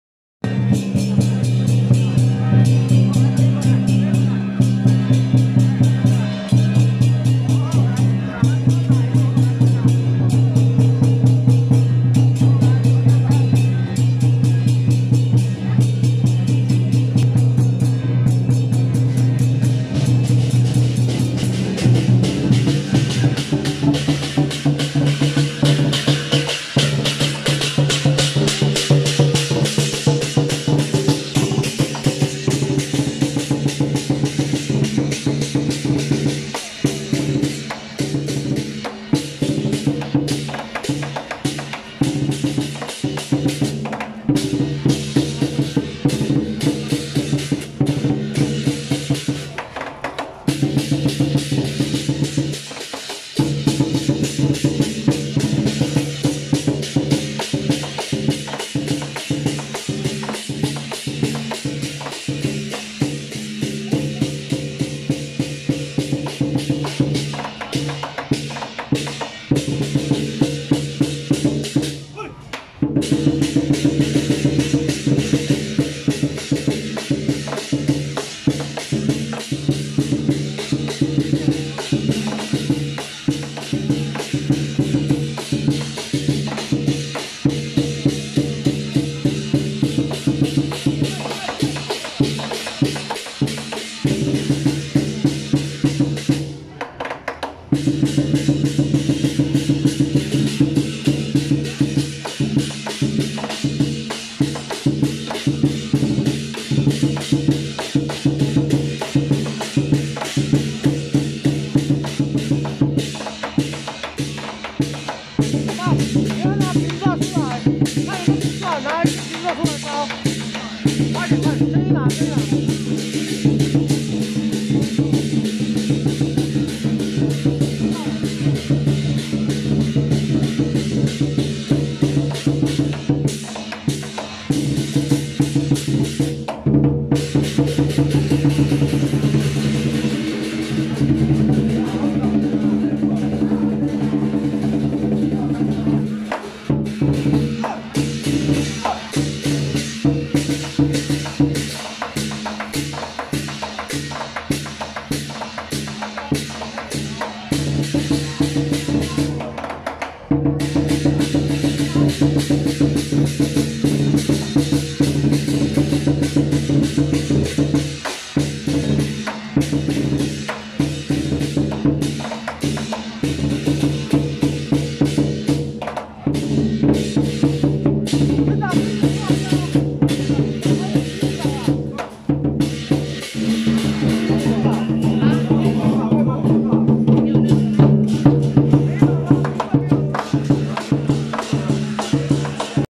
【臺北鼓舞坊-醒獅團】~108士林湳雅福安宮永安社恭迎天上聖母遶境
臺北鼓舞坊-醒獅團